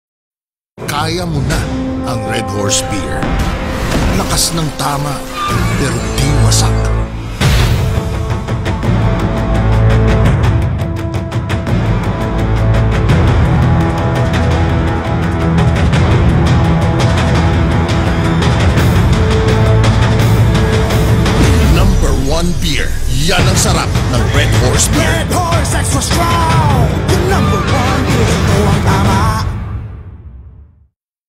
Red Horse Beer 2015
TV Commercial for Red Horse Beer with Sol Mercado.